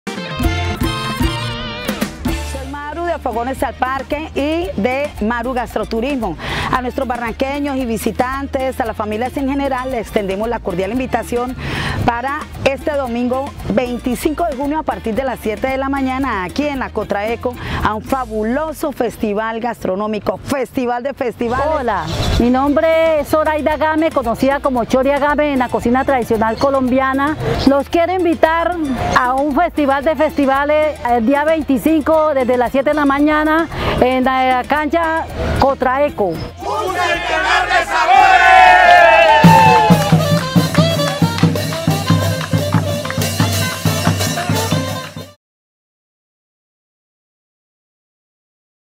Invitación al festival "Centenar de sabores" este 25 de junio en la cancha Cotraeco
Información cultural: este próximo domingo más de 30 expositores gastronómicos de Barrancabermeja y la región se darán cita en la cancha Cotraeco en el festival “Centenar de sabores”. A continuación, la invitación que hacen María Eugenia Artunduaga y ‘La Chori Agámez’, cocineras tradicionales del distrito.